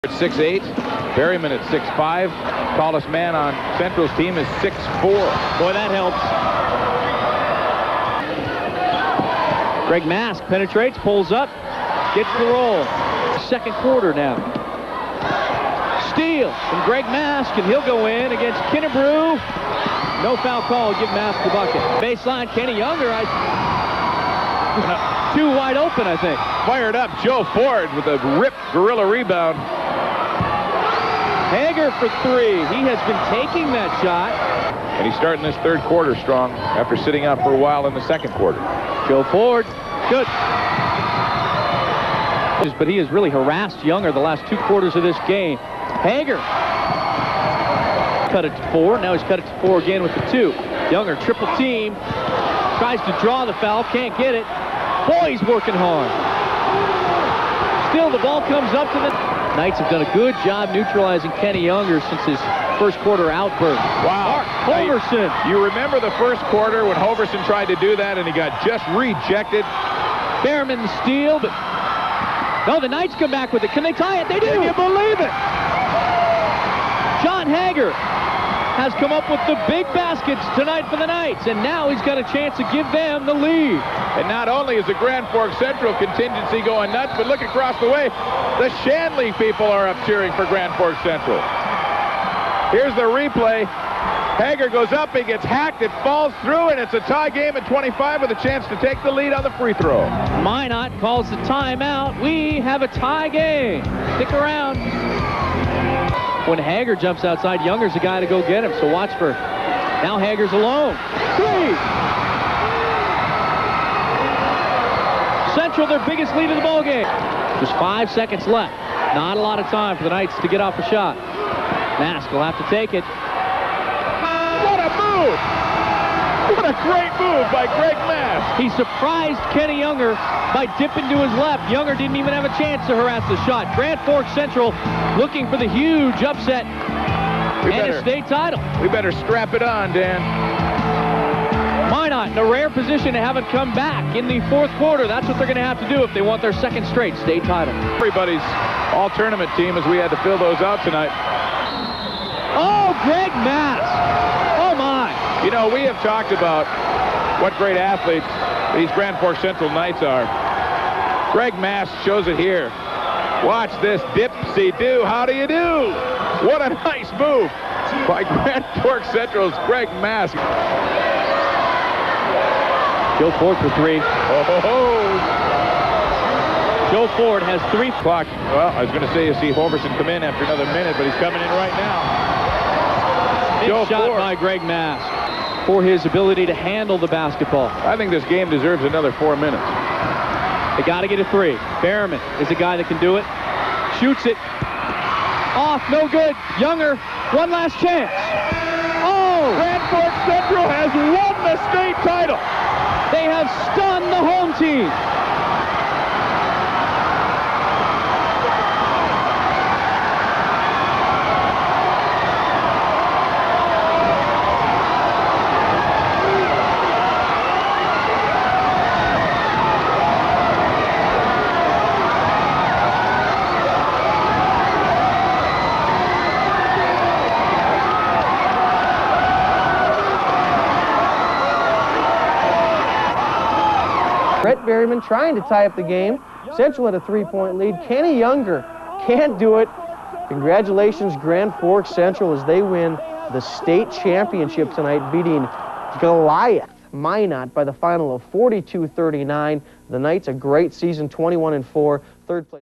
Grand Forks Central Highlights vs Minot High - North Dakota State Basketball Title Game (1996)
Grand Forks Central Knights highlights vs the Minot High Magicians in the 1996 North Dakota Class A Championship game.